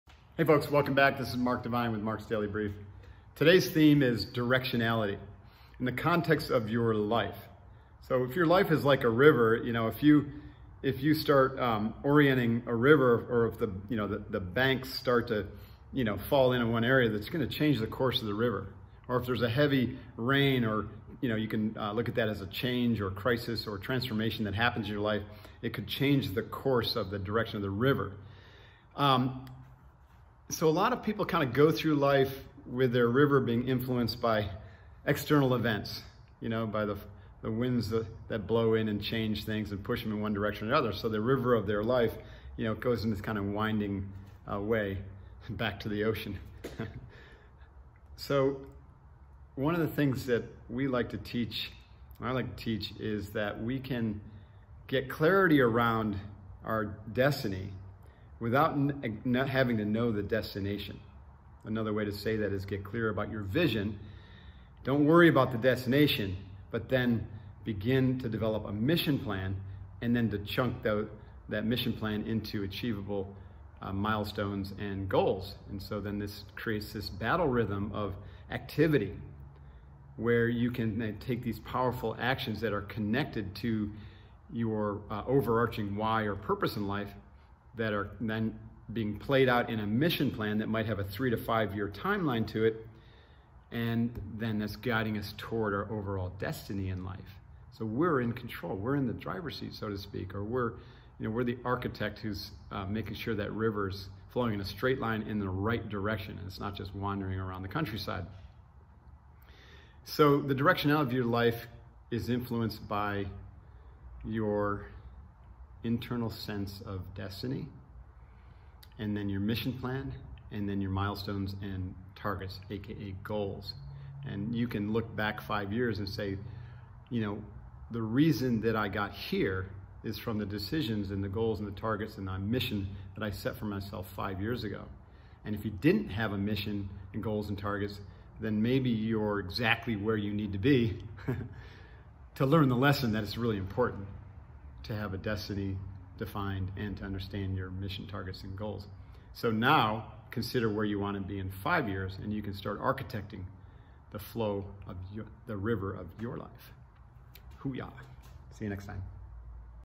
Mark's Daily Brief: Directionality
Your life is like a river. If the banks start to fall-in in one area or a heavy rain falls, it could change the course of the direction of the river. A lot of people go through life with their ‘river’ being influenced by external events. We can get clarity around our destiny without having to know the destination. In other words, get clear about your vision and develop a mission plan to chunk your goals down into manageable targets so that you can ensure you are guiding yourself towards your destiny in life. Directionality in life is influenced by your internal sense of destiny, milestones, and goals. The reason you are here today is because of the decisions, targets, and goals you set five years ago. Now, consider where you want to be in five years and begin architecting the flow of the river in your life. Stay focused 👊